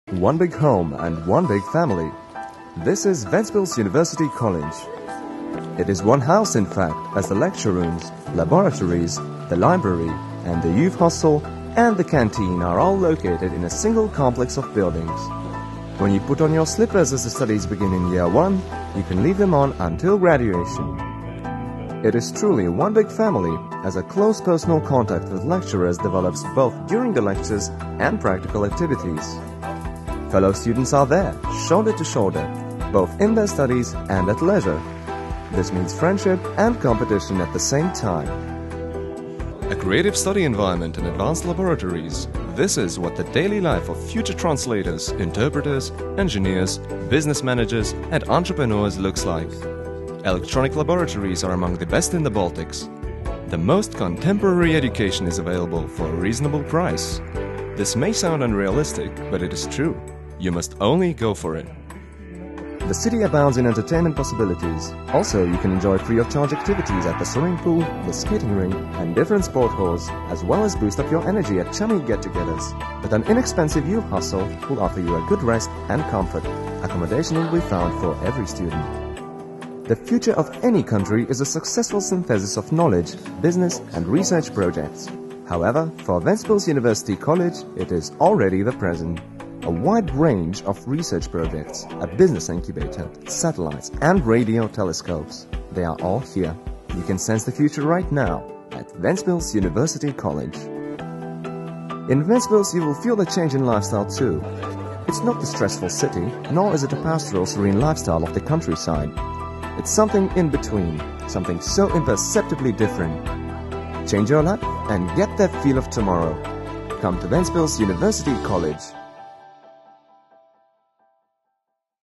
Ventspils Augstskola (Ventspils University College)
Ventspils University College was founded in 1997. It is a self-governing state university college and scientific institution and its basic activity is to carry out scientific research and to implement academic and professional study programmes. Ventspils University College offers to achieve knowledge in 3 faculties and 10 study programmes in the following specialties: business management, translation/interpreting and linguistics, computer science and engineering.